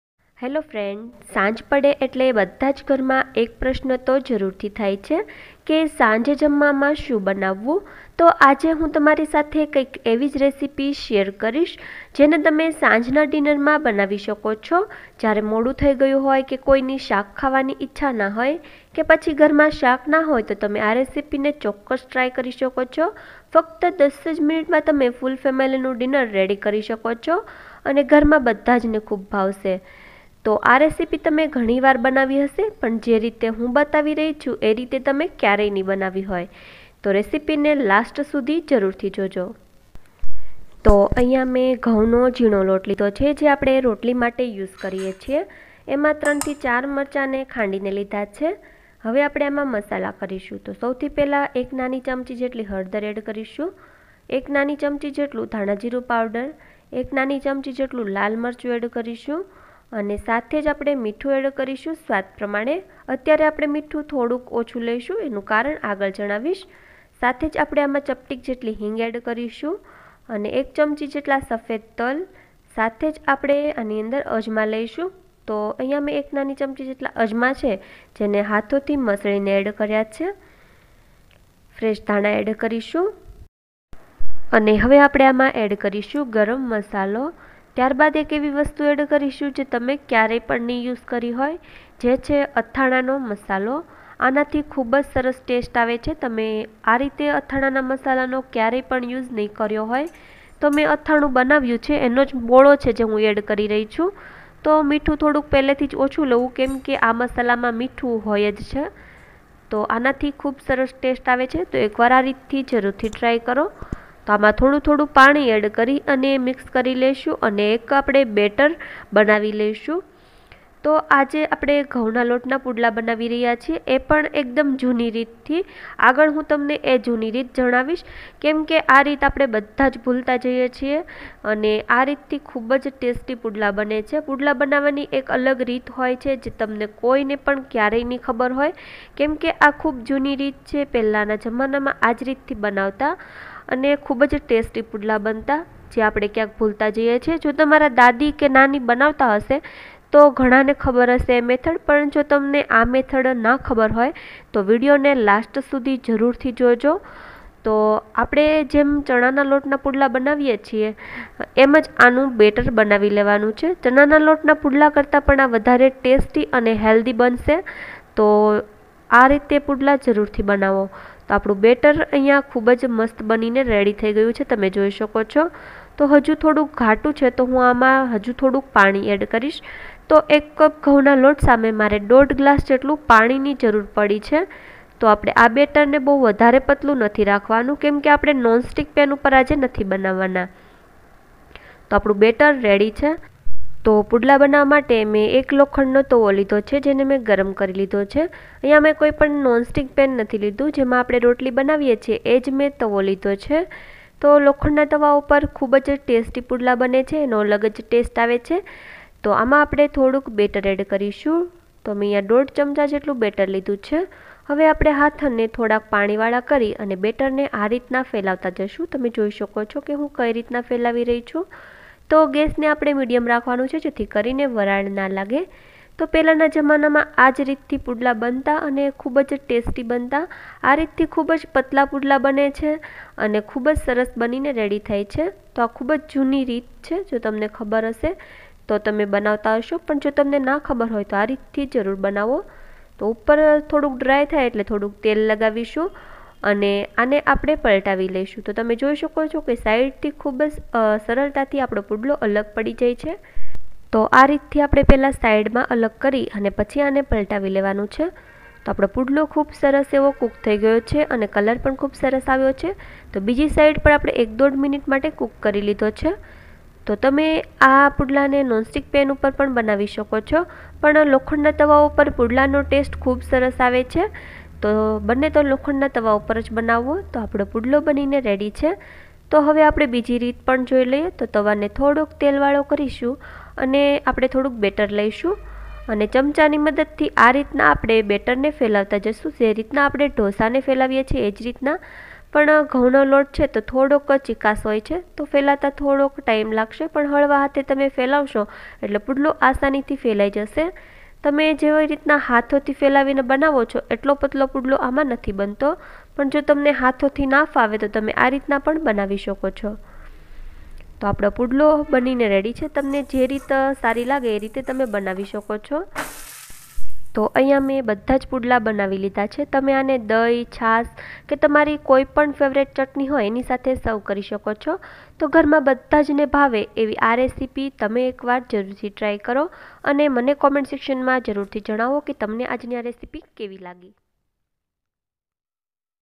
સાંજ માટે બેસ્ટ ડિનર ઓપ્શન || ૧૦ જ મીનીટમાં બનાવો ફુલ ફેમીલી નું ડિનર એ પણ જુની રીત થી
એક એવી રેસીપી જે ઘરમાં નાના મોટા અને ધરડા બધાજ ને ખુબ જ ભાવે...અને આ રેસીયી તમે એકવાર બનાવશો તો બીજીવાર બનાવીને ખાવાની પંસદ કરશો ..કેમકે આ ખુબજ ટેસ્ટી હોવાની સાથે આને બનાવવી પણ ખુબજ સરળ છે. તો એકવાર આ રેસીપી જરૂર બનાવજો

best gujarati dinner idea.
this is my all time Favorite dinner recipe.
it's helthy and easy to cook.